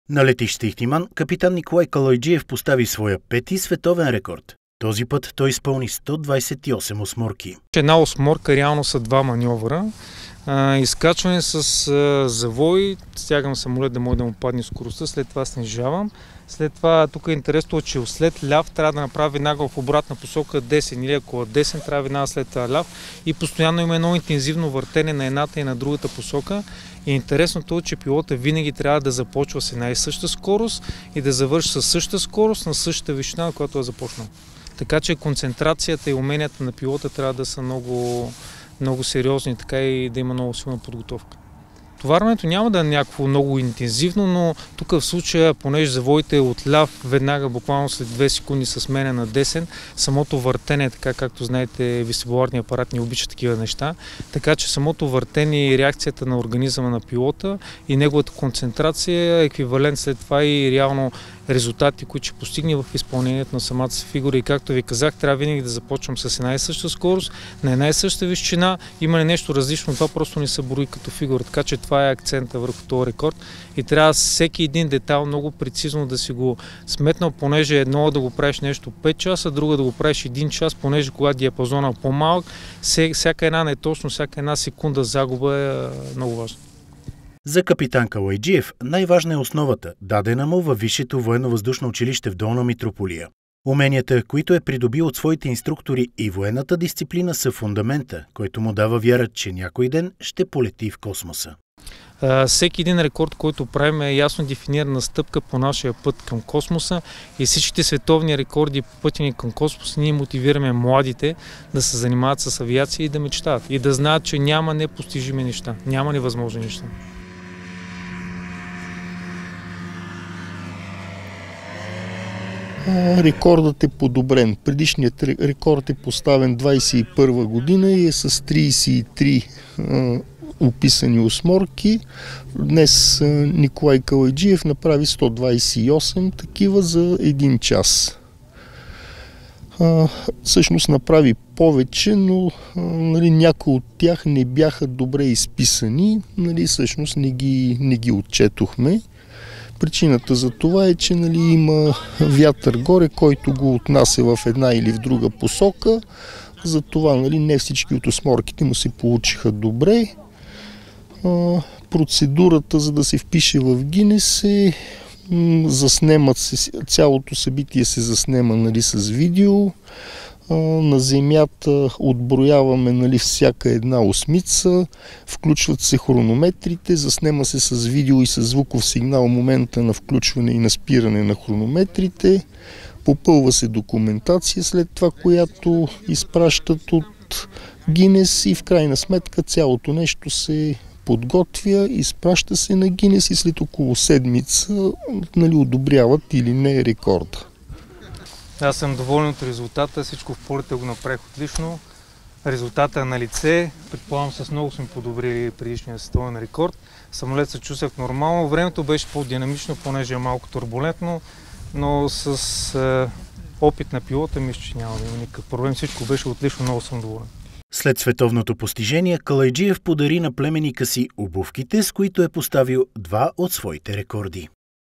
Пети световен рекорд за капитан Калайджиев
Световен рекорд за капитан Николай Калайджиев. Той счупи поредния си рекорд на Гинес, постижението е за „Най-много осморки изпълнени за един час“. Калайджиев направи 128 маневри подобрявайки многократно досегашното постижение от 33 осмици на американски пилот от 2021 г.